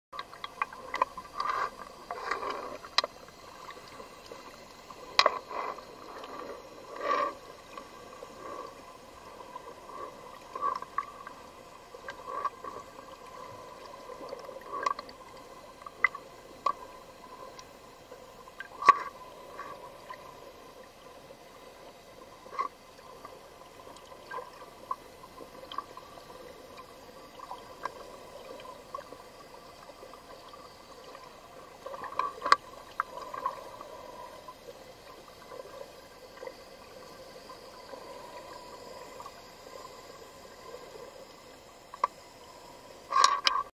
Devil Ray in Grand Case, Saint Martin, filmed by Octopus Diving!
This is a 5 foot wide "Mobula" or Devil Ray swimming up and down Grand Case bay feeding on juvenile fish. This Video was shot by Chris Davies in 4 feet of water while snorkeling.